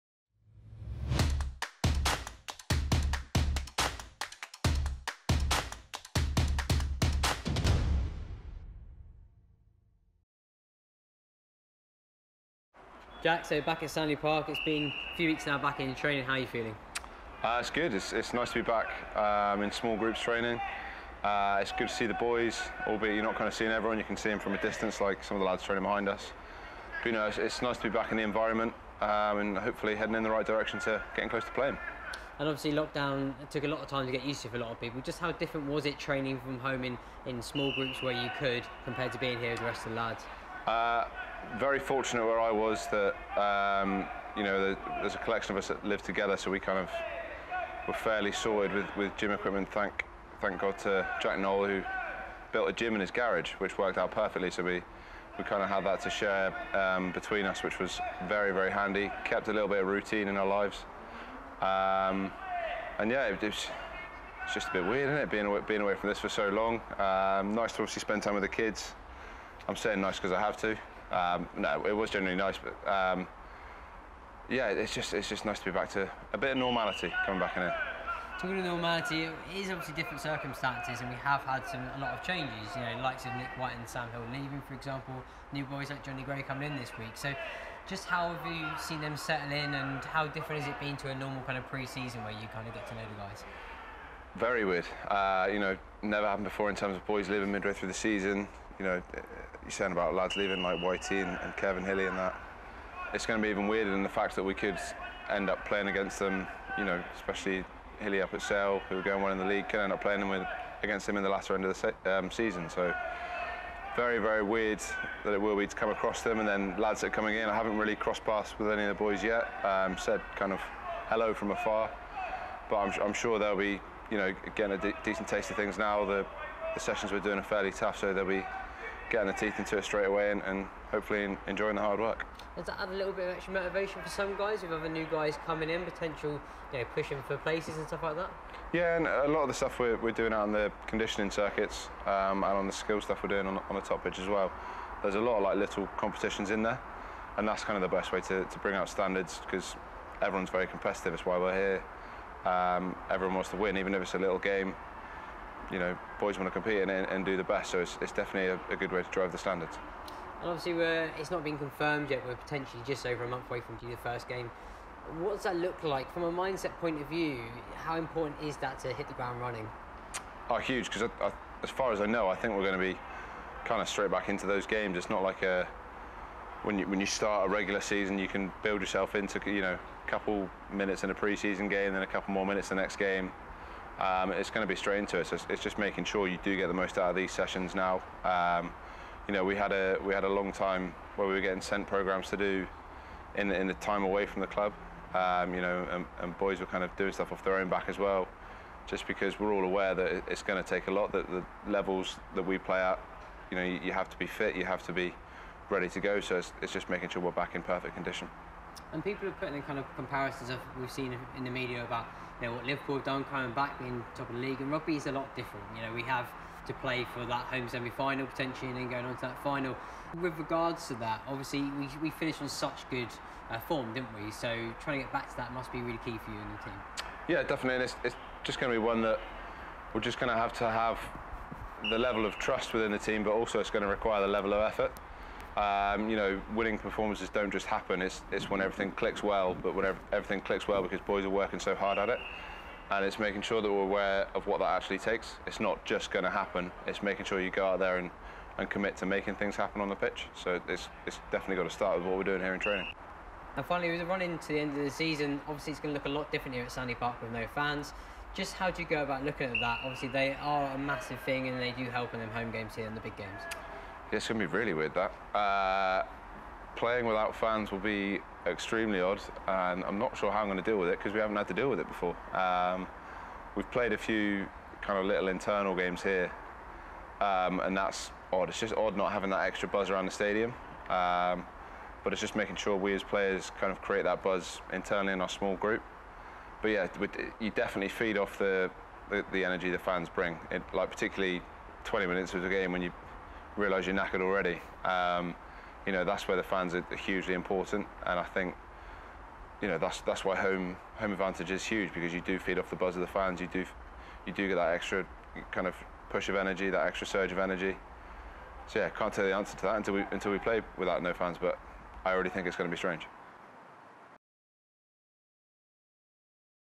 Chiefs TV - Jack Yeandle on Rugby Restart Training
Chiefs TV - We speak with Club Captain Jack Yeandle on all things restart training, including home workouts, missing being around the club, and how Exeter Chiefs prepare to go back into battle.